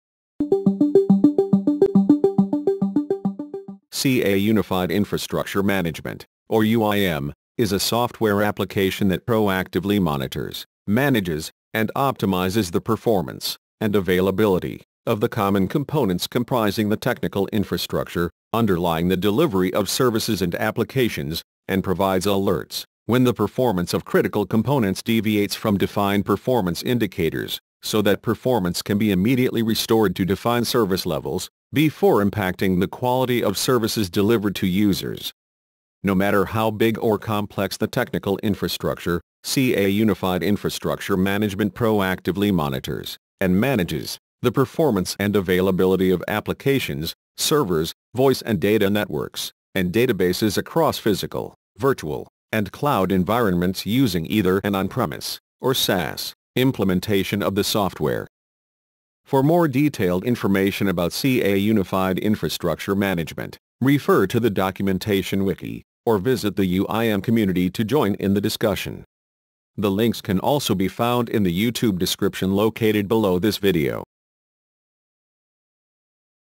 CA UIM What it Does
This video highlights CA Unified Infrastructure Management (CA UIM) functionality and components. For more detailed information about CA UIM, refer to the documentation wiki or visit the UIM community to join in the discussion!